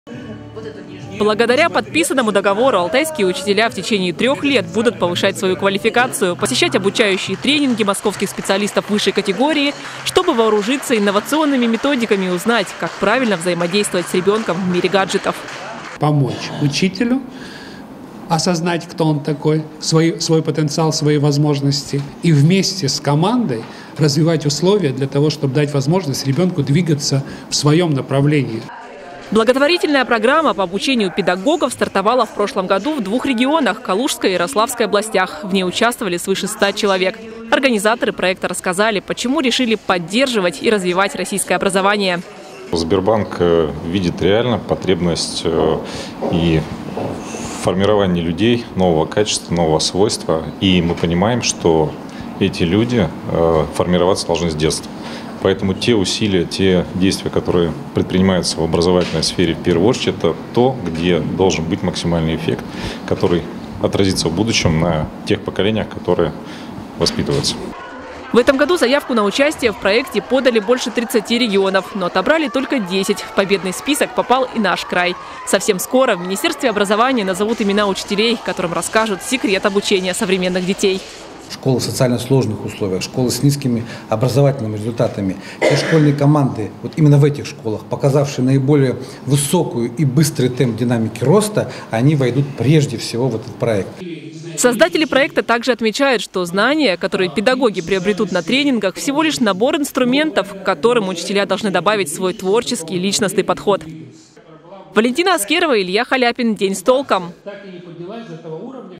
Алтайских учителей обучат инновационным методикам работы с детьми нового поколения
Нестандартным подходам в обучении школьников, работе в команде - этому научат алтайских педагогов в рамках образовательной программы, которую реализует благотворительный фонд Сбербанка «Вклад в будущее».  Министерство образования края и представители банка подписали договор о сотрудничестве.